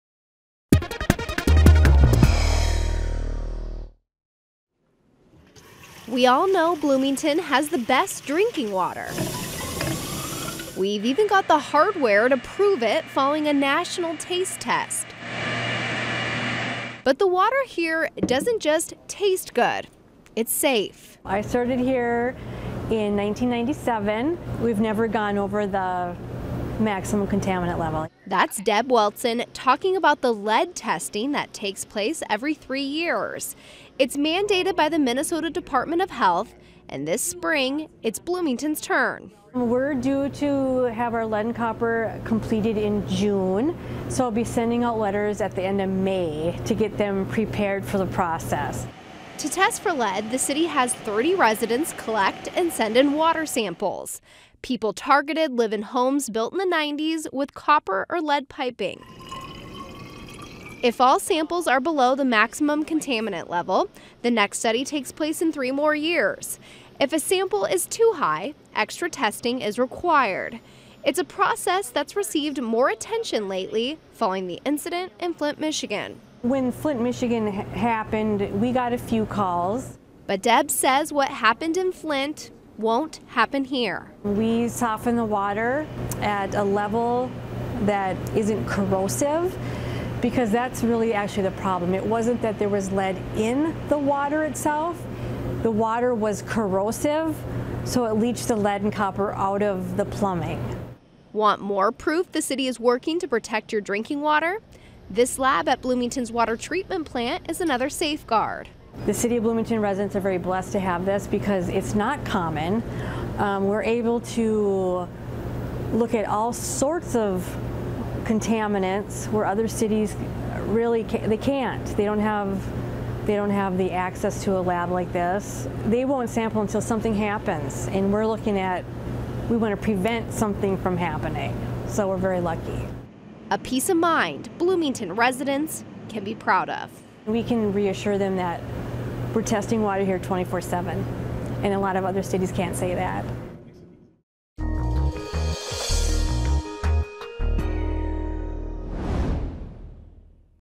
How Does The City Test for Lead in Water?
Water Quality Supervisor Deb Weltzin explains how the City tests for lead in drinking water and how the lab at the treatment plant protects from other contaminants.